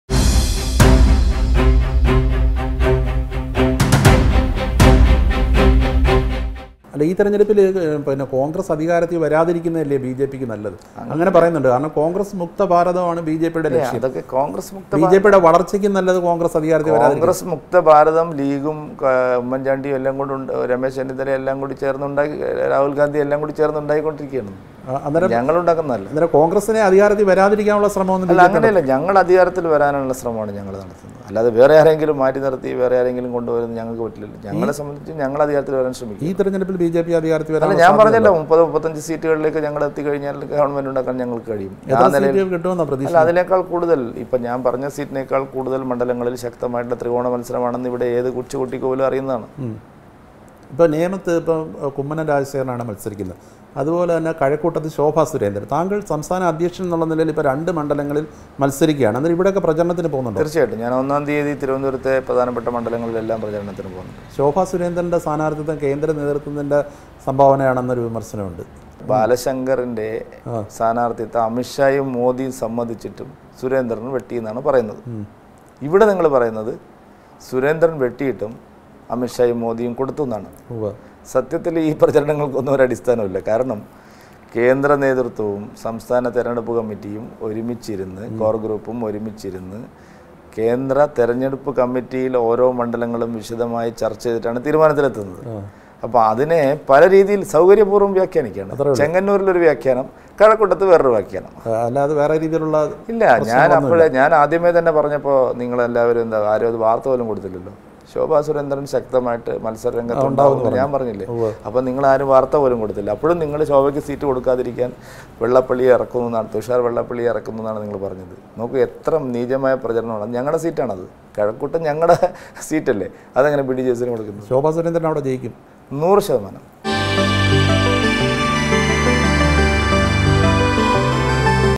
Had K Surendran cut the candidature of Shobha Surendran? | Straight Line
K. Surendran is an Indian politician from the state of Kerala. He is one of the general secretaries of the Bharatiya Janata Party in the state.K Surendran assumes charge as Kerala BJP state chief